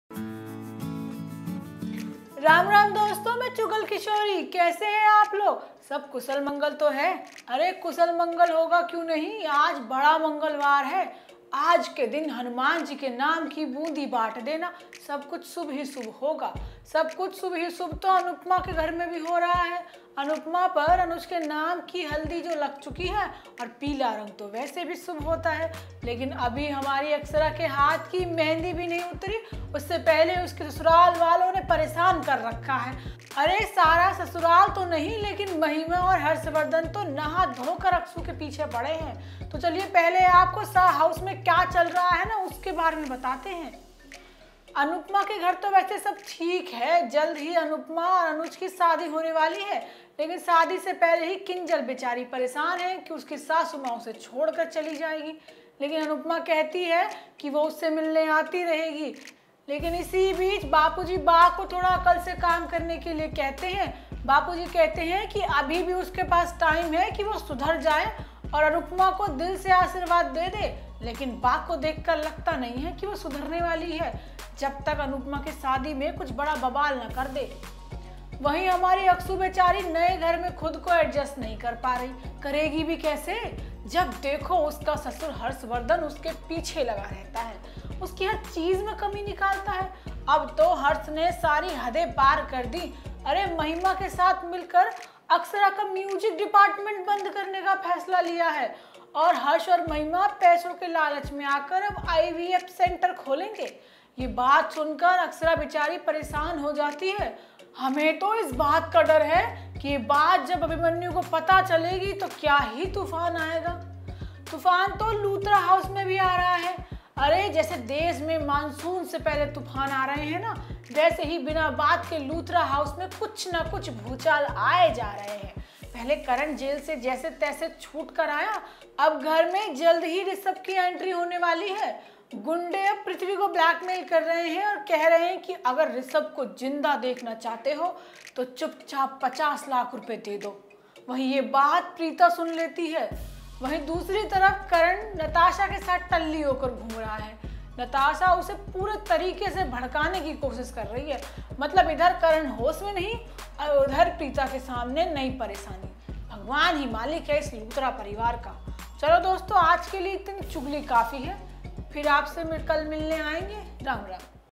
EP016| Chugalkishori: बंद होगा अक्षरा का म्यूजिक डिपार्टमेंट तो कुंडली भाग्य में होगी ऋषभ की एंट्री
EP016| Chugalkishori: राम राम दोस्तों...मैं चुगल किशोरी...कैसे हैं आप लोग..सब कुशल मंगल...अरे कुशल मंगल होगा क्यों नहीं होगा. आज बड़ा मंगलवार जो है। आज के दिन हनुमान जी के नाम की बूंदी बांट देना...सब शुभ ही शुभ होगा।...सब कुछ शुभ ही शुभ तो अनुपमा के घर में भी हो रहा है। अनुपमा पर अनुज के नाम की हल्दी जो लग चुकी है और पीला रंग तो वैसे ही बहुत शुभ माना जाता है...लेकिन अभी हमारी अक्षरा की हाथ की मेहंदी भी नहीं उतरी कि उससे पहले ही उसके ससुराल वालों ने उसको परेशान कर रखा है।अरे सारा ससुराल तो नहीं...लेकिन महिमा और हर्षवर्धन तो नहा-धोकर अक्षु के पीछे पड़े हैं..तो चलिए पहले आपको शाह हाउस में क्या चल रहा है उसके बारे में बताते हैं।